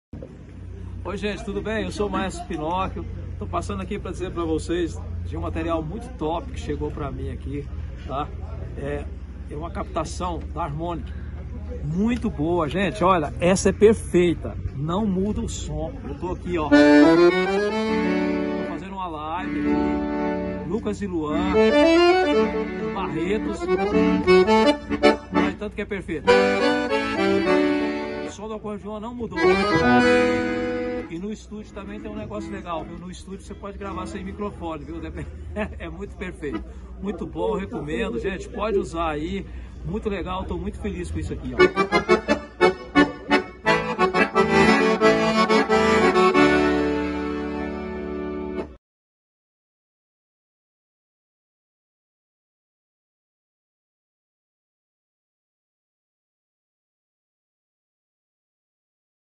Maestro Pinocchio - Harmonik Microphones
Um orgulho pra Harmonik ter esse ícone do acordeon e da música sertaneja usando nossa captação. Obrigado pela confiança Maestro Pinocchio 🎹🙌